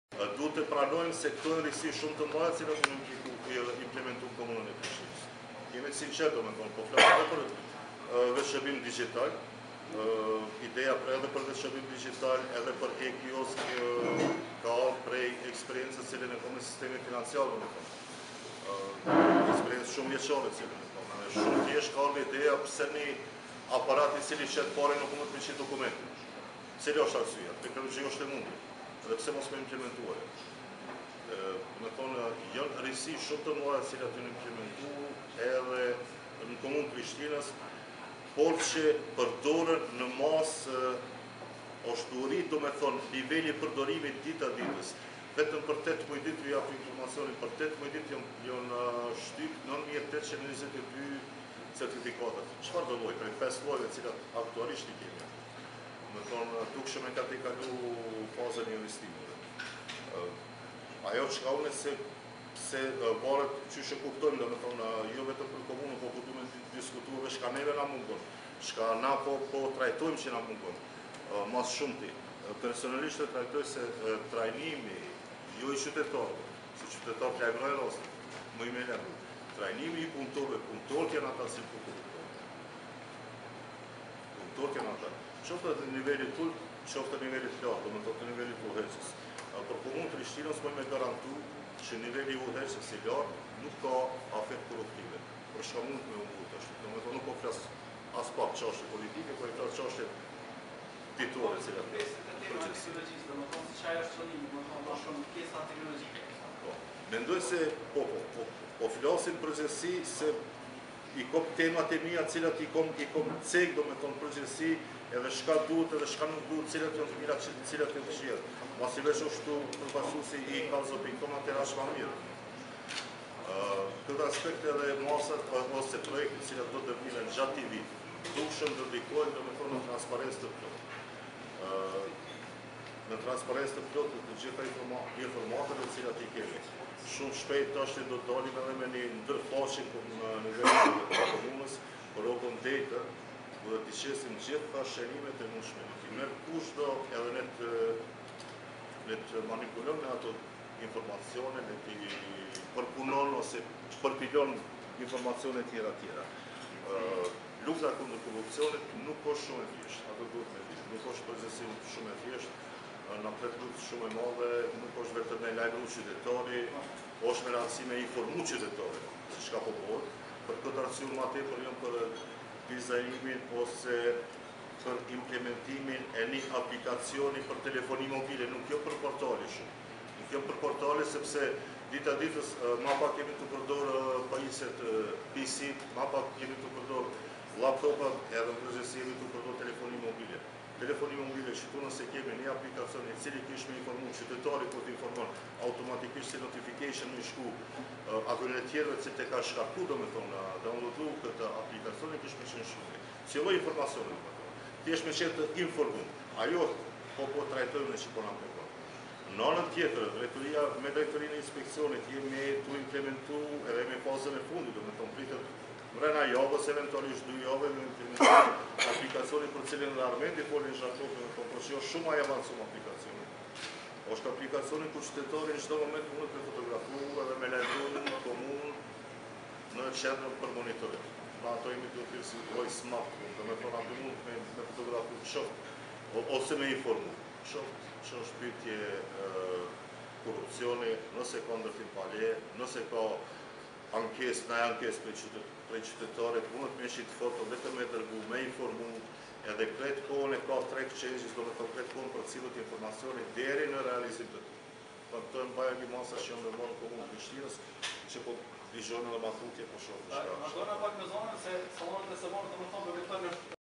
Muhedin Nushi në Sallonet e së martes
Muhedin Nushi, Drejtor i Drejtorisë së Administratës, Komuna e Prishtinës,flet për temën:Teknologjia kundër korrupsionit: Si mundet një uebfaqe apo aplikacion t’i fuqizojë qytetarët?